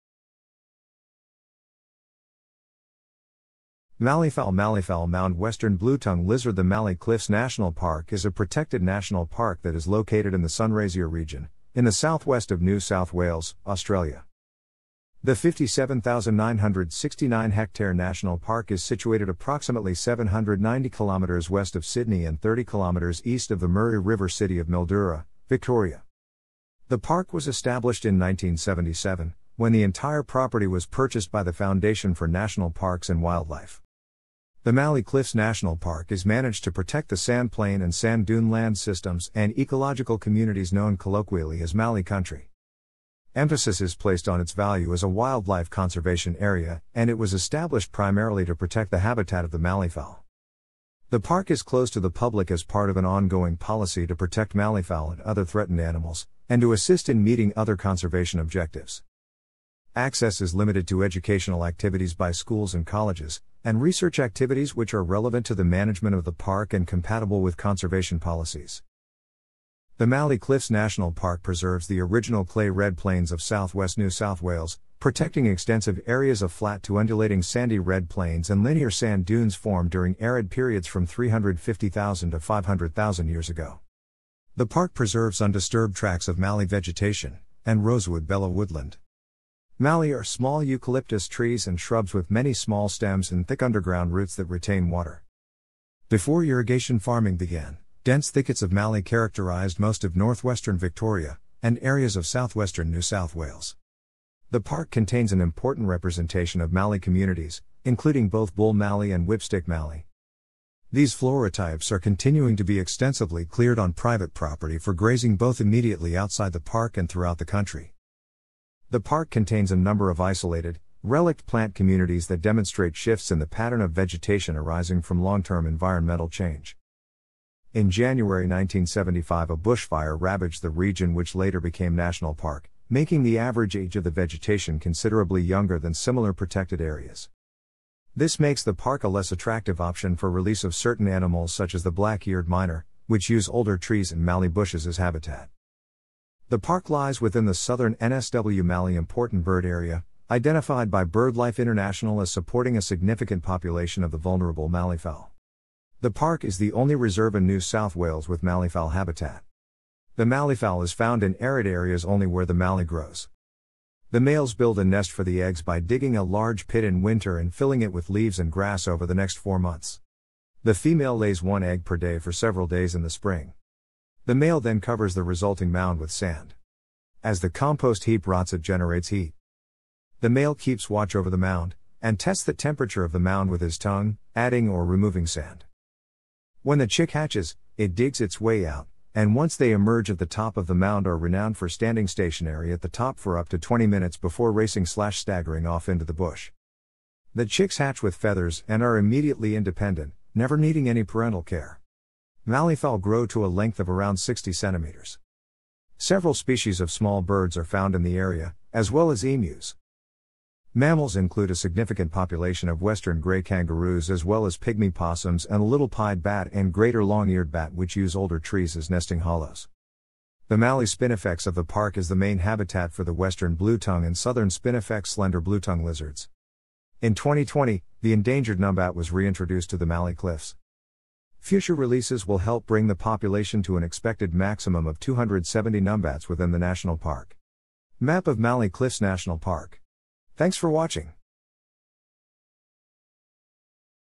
Mallee Cliffs National Park
Malleefowl Malleefowl mound Western blue-tongue lizard The Mallee Cliffs National Park is a protected national park that is located in the Sunraysia region, in the south-west of New South Wales, Australia.
The 57,969-hectare （143,240-acre） national park is situated approximately 790 kilometres （490 mi） west of Sydney and 30 kilometres （19 mi） east of the Murray River city of Mildura, Victoria.
The park was established in 1977, when the entire property was purchased by the Foundation for National Parks & Wildlife.
The Mallee Cliffs National Park is managed to protect the sand plain and sand dune land systems and ecological communities known colloquially as Mallee Country.
Emphasis is placed on its value as a wildlife conservation area and it was established primarily to protect the habitat of the malleefowl.
The park is closed to the public as part of an ongoing policy to protect malleefowl and other threatened animals, and to assist in meeting other conservation objectives.
Access is limited to educational activities by schools and colleges, and research activities which are relevant to the management of the park and compatible with conservation policies.
The Mallee Cliffs National Park preserves the original clay red plains of south west New South Wales, protecting extensive areas of flat to undulating sandy red plains and linear sand dunes formed during arid periods from 350,000 to 500,000 years ago.
The park preserves undisturbed tracts of mallee vegetation, and rosewood-belah woodland.
Mallee are small eucalyptus trees and shrubs with many small stems and thick underground roots that retain water.
Before irrigation farming began, dense thickets of mallee characterized ...